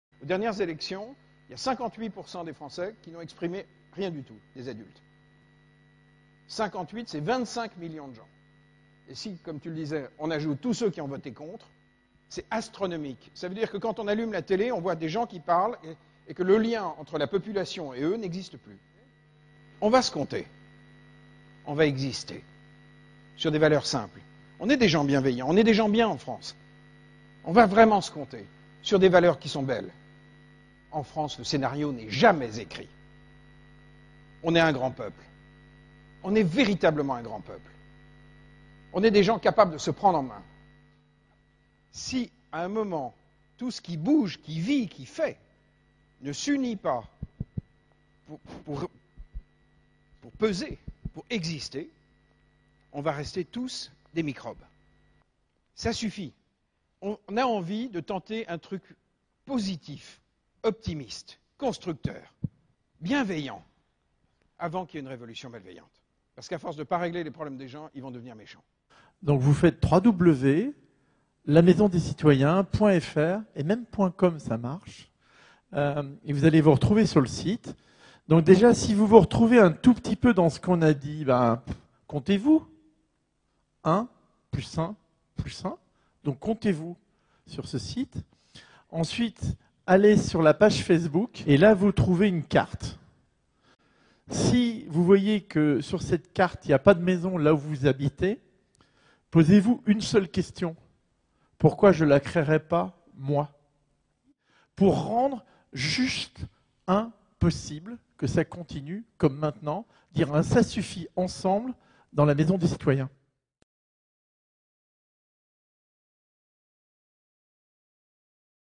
La Maison des Citoyens, une réponse bienveillante... tant qu'il en est encore temps !
Alexandre Jardin et Robert Branche à Calais
Conférence RIVRH : Pourquoi la Maison des Citoyens